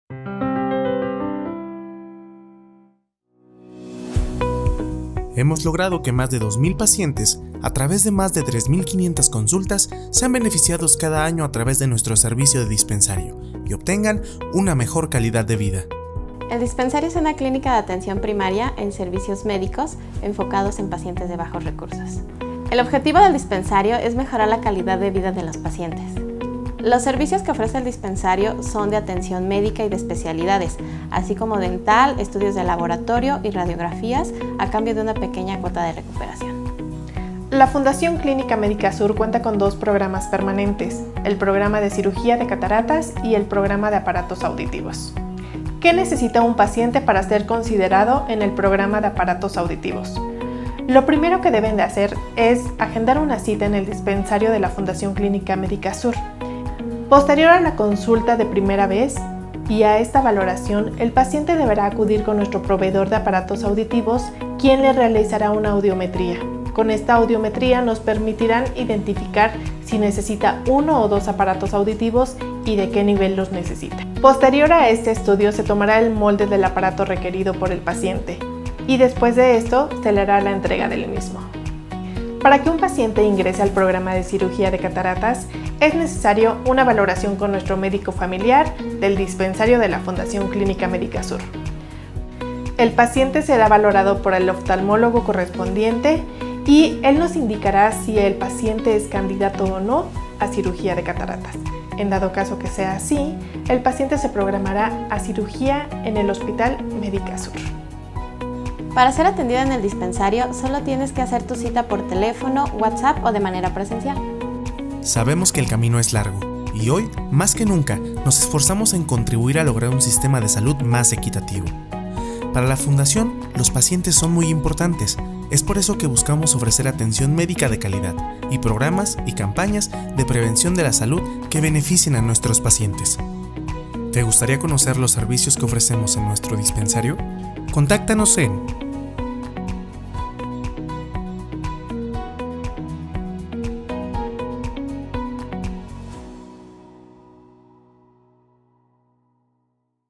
Fundación Clínica Médica Sur - Dispensario
Sabías que la @Fundación Clínica Médica Sur a través del Dispensario cuenta de forma permanente con los servicios de consulta general, 20 especialidades, nutrición y psicología, así como el Programa de Cirugía de Cataratas y el Programa de Aparatos Auditivos.
¡Conoce más en nuestra página web y agenda tu cita al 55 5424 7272!